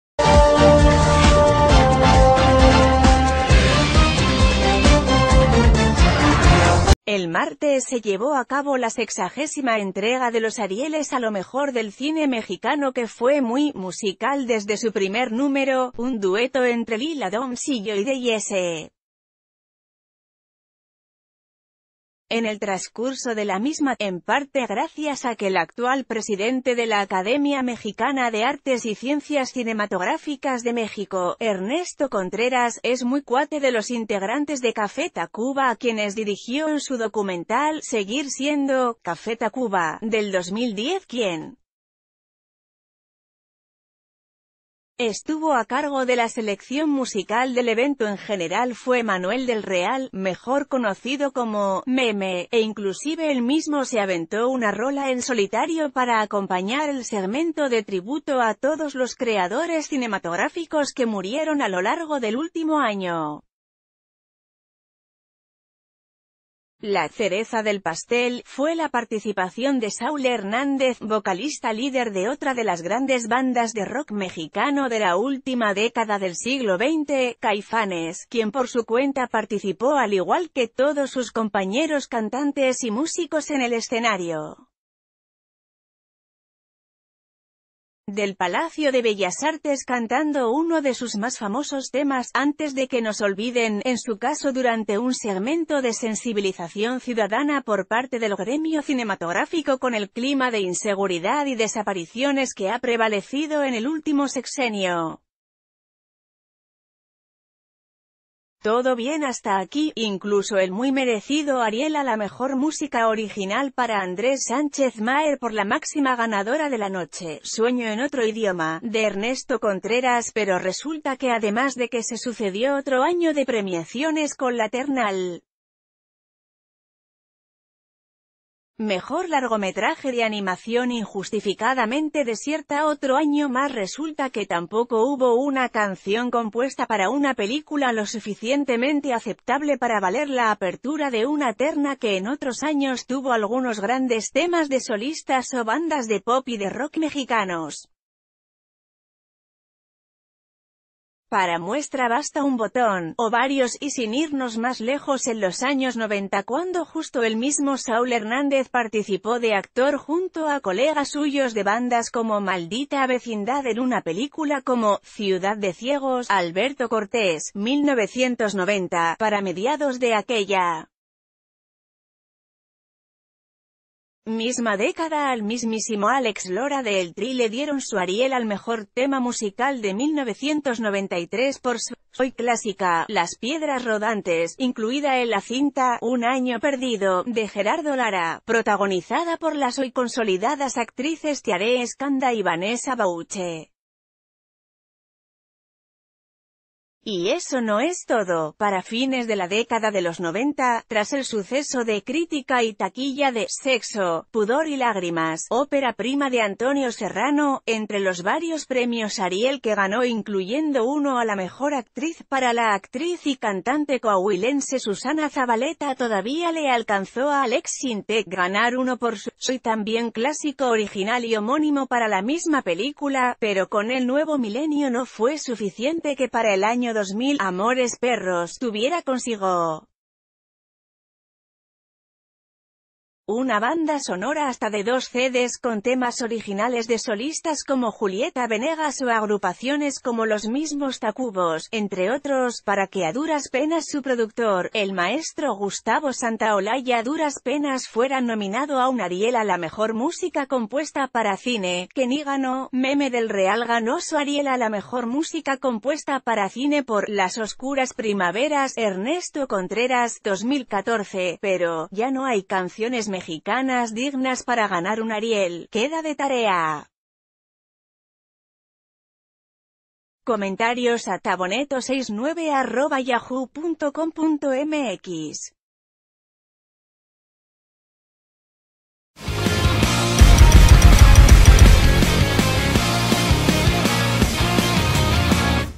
¿Ya no hay canciones dignas de un Ariel?
El martes se llevó a cabo la Sexagésima Entrega de los Arieles a lo mejor del cine mexicano que fue muy musical desde su primer número, un dueto entre Lila Downs y Joy de Jesse & Joy. En el trascurso de la misma, en parte gracias a que el actual Presidente de la Academia Mexicana de Artes y Ciencias...